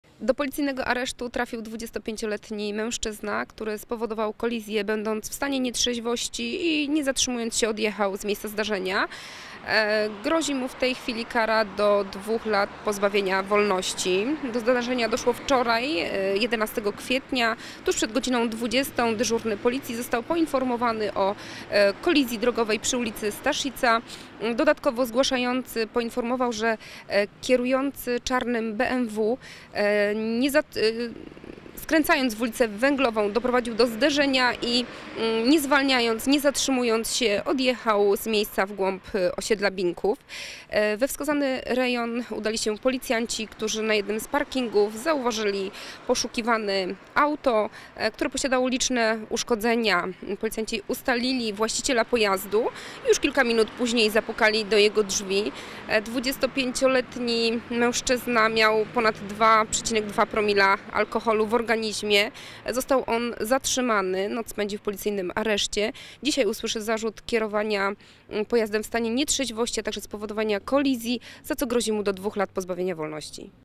TKB - Pijany uciekł z miejsca zdarzenia - 12.04.2018
Pijany 25-latek swoim BMW spowodował na ulicy Węglowej kolizję i nawet nie zatrzymał się, by upewnić czy nie trzeba nikomu udzielić pomocy.

Dane mężczyzny szybko ustalili policjanci. Odnaleźli zaparkowany poobijany samochód i pijanego kierowcę. Teraz grozi mu do 2 lat pozbawienia wolności.